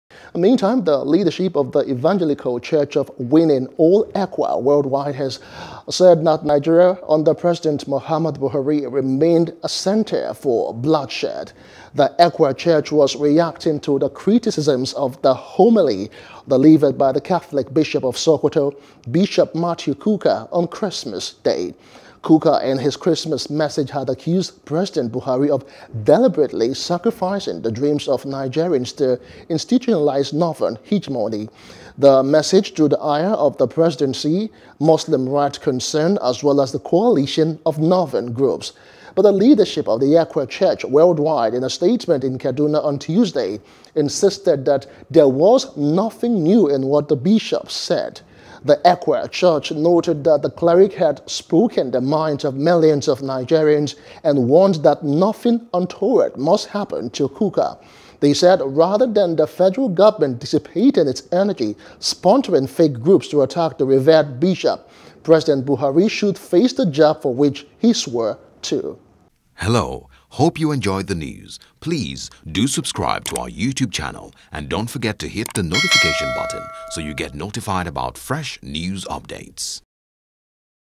Kukah’s Message: Nepotism Under Pres. Buhari is Top-Notch – ECWA (NEWS | NIGERIA)
Kukah’s Message: Nepotism Under Pres. Buhari is Top-Notch – ECWA